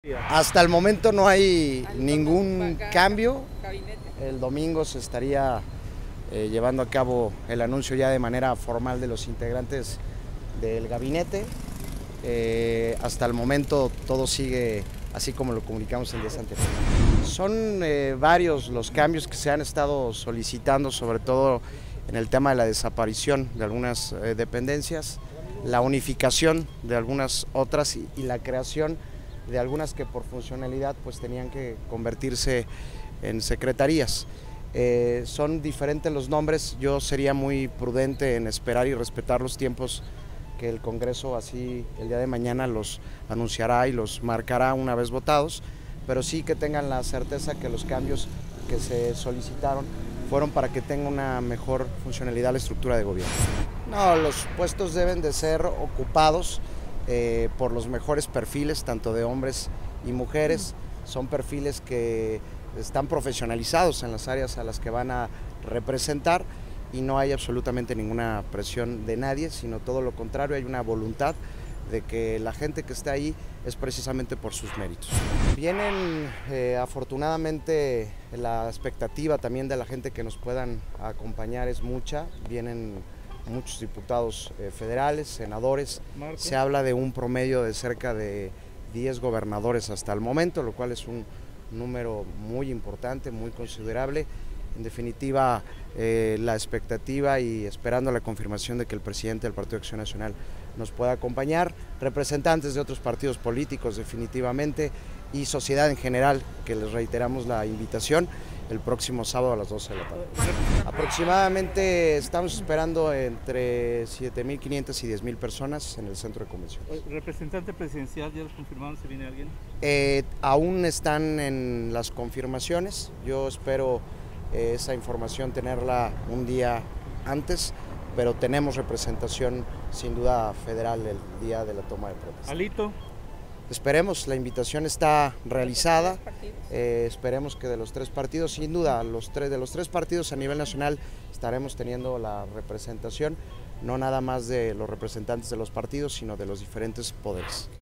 No hay cambios: Gabinete estatal se conocerá hasta el domingo: Kike de la Torre
* Quienes vayan al Gobierno Estatal lo harán por capacidad, no por presiones
* Esperan la presencia de al menos 10 gobernadores, funcionarios y dirigentes de partido a la toma de protesta. Hasta Alito está invitado

Aguascalientes, Ags. 28 de septiembre 2022.- A pesar de las oleadas de especulaciones en torno a los presuntos integrantes del gabinete con que la gobernadora Tere Jiménez Esquivel arrancará su gobierno, no hay cambios y será hasta el domingo cuando la propia mandataria estatal dé a conocer la conformación oficial de su gabinete, reiteró el Vocero del Gobierno Estatal electo, Kike de la Torre.
En entrevista colectiva, expuso que la selección de colaboradores de Tere Jiménez, se llevará a cabo sin presiones y tomando en cuenta de manera primordial la capacidad, profesionalismo, méritos y capacidad de trabajo.
Expuso que aún es necesario esperar la determinación que mañana jueves tome el pleno del Congreso del Estado, sobre los cambios solicitados en torno a la confirmación de varias dependencias y organismos, así como la creación de algunas más.
Finalmente Kike de la Torre aseguró que, durante el evento central de arranque de Gobierno, se espera la presencia de al menos 10 gobernadores, diputados federales, senadores y los dirigentes nacionales de los partidos coligados, incluido Alito Moreno. Sobre el representante presidencial, aún no se tiene certeza de quien acudirá.